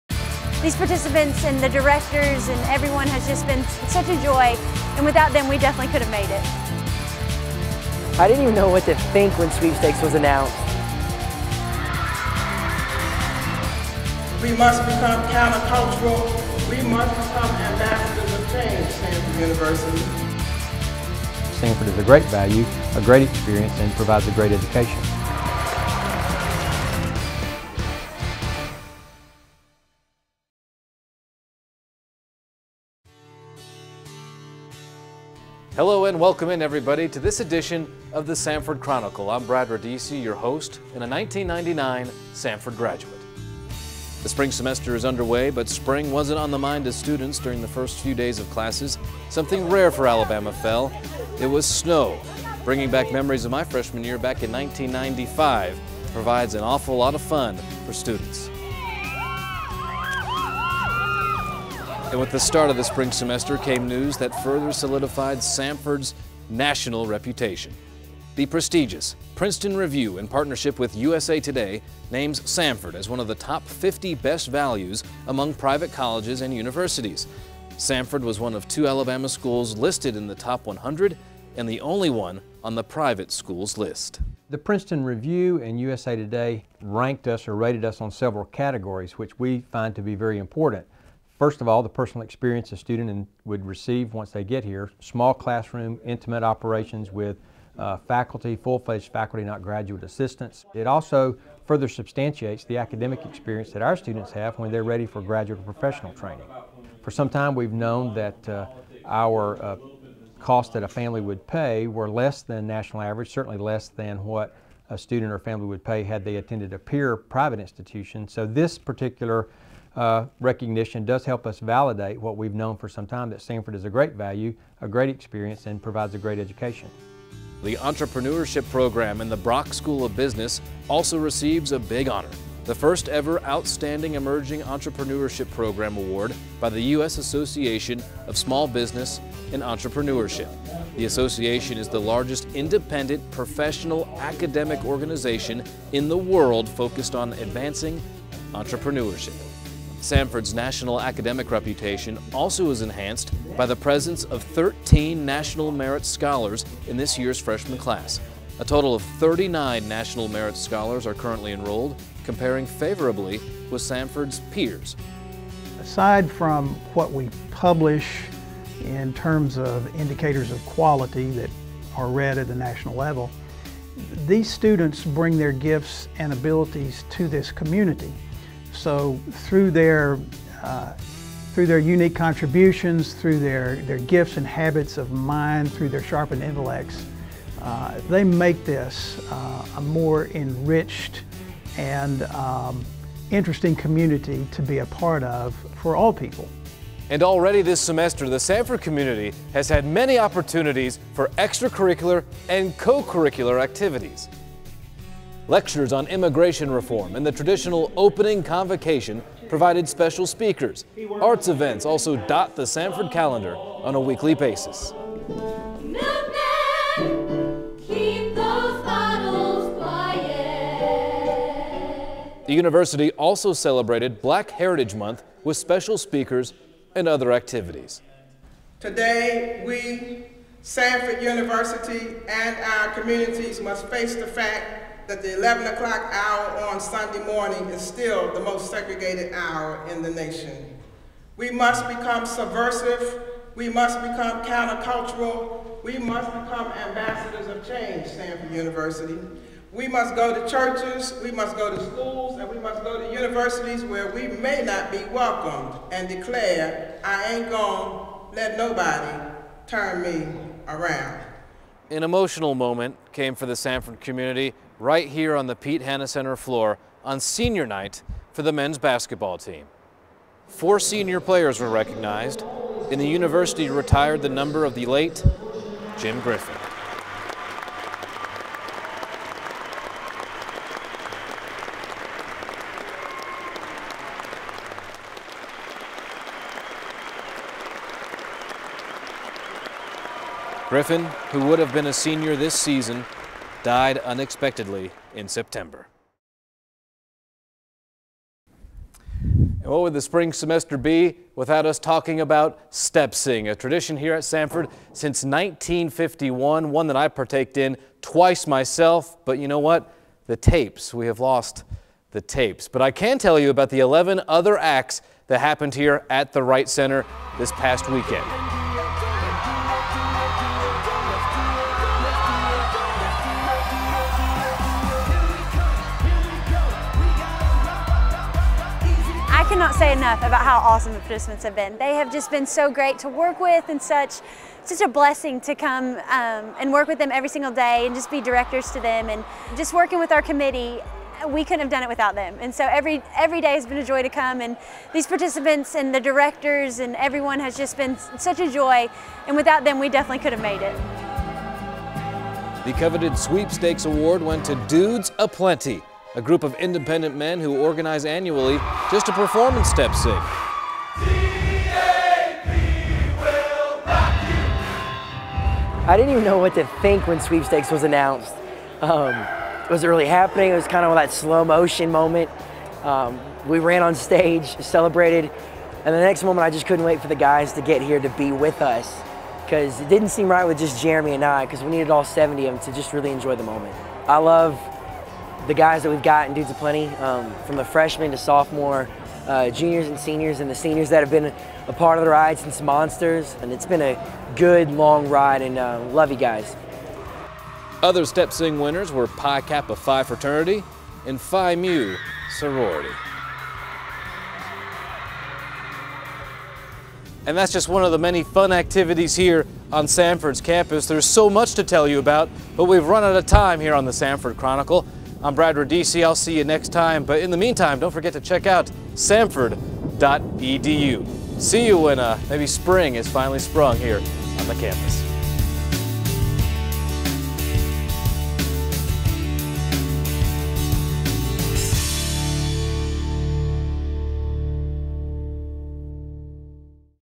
The Samford Chronicle: February Issue
This edition of The Samford Chronicle covers some recent awards for Samford, the start of a new semester, and, of course, Step Sing.